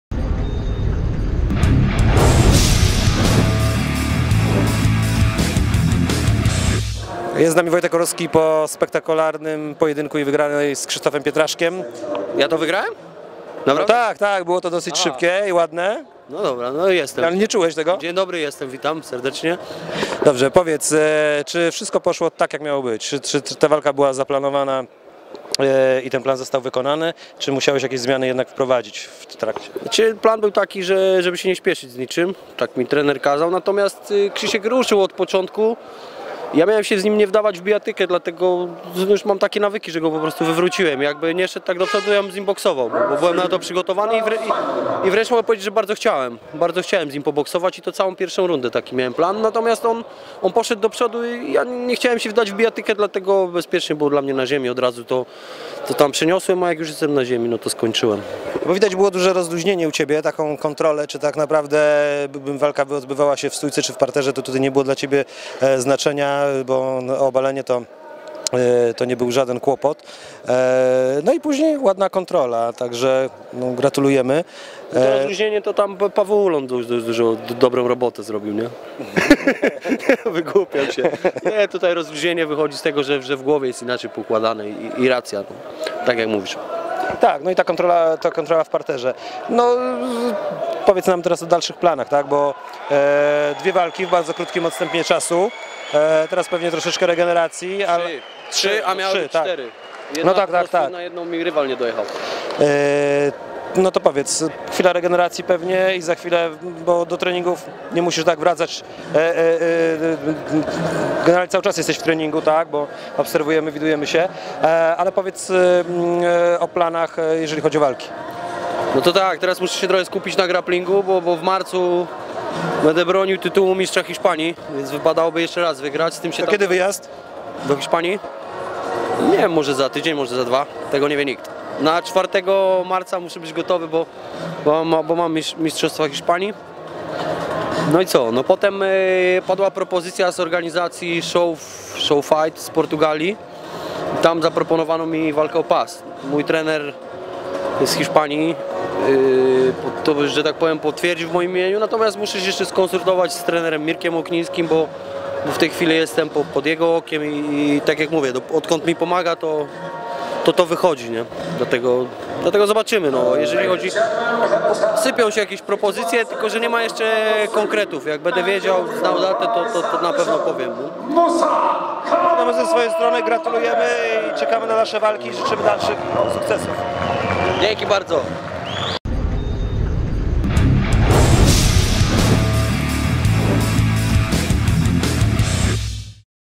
WOJCIECH ORŁOWSKI po wygranej na ACB29 Torwar [wywiad] - TVPitBullWarszawa
Wojciech Orłowski, zawodnik Nastula Club zaraz po wygranej z Krzysztofem Pietraszkiem na ACB29 Torwar. Trochę o walce oraz planach na przyszłość.

video/montaż: Aleksandra Sędek
wywiad: Paweł Ciastek